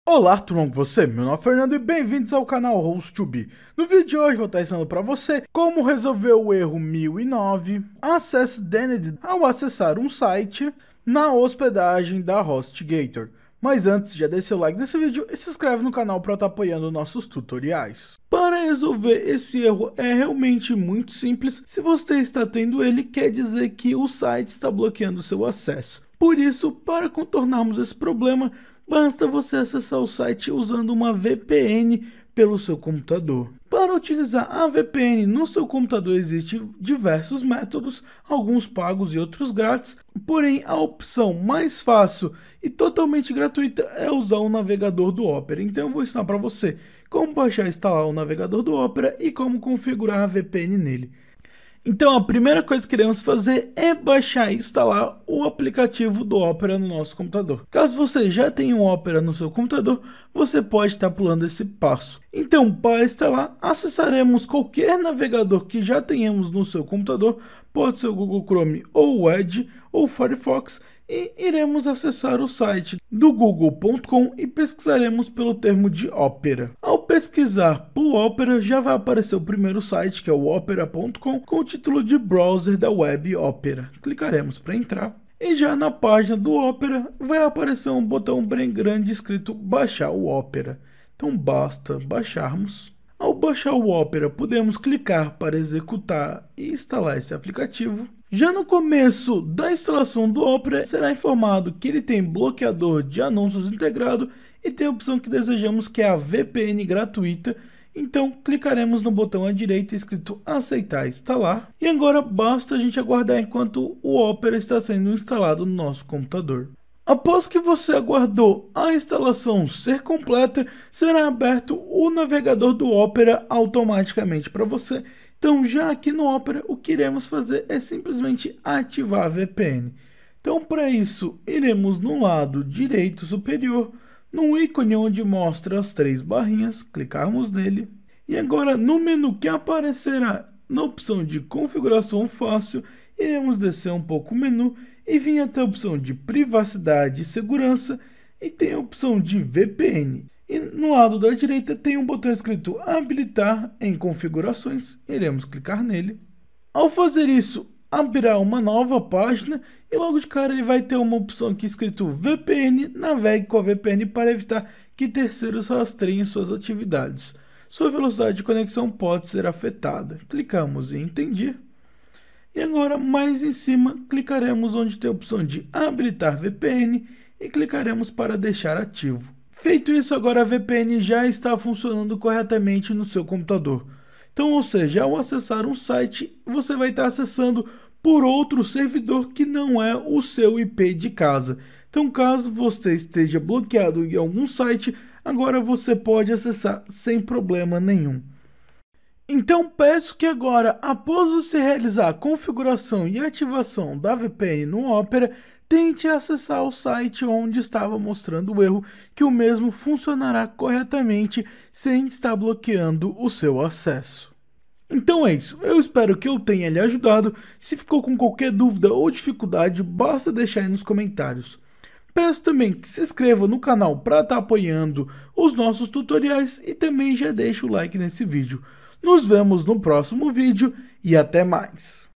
COMO RESOLVER Erro 1009 Access Denied na Hostgator (Rápido e Fácil) 2022
Aprenda Como Resolver o erro 1009 Access Denied na Hostgator facilmente e em poucos minutos

LINKS IMPORTANTES!!!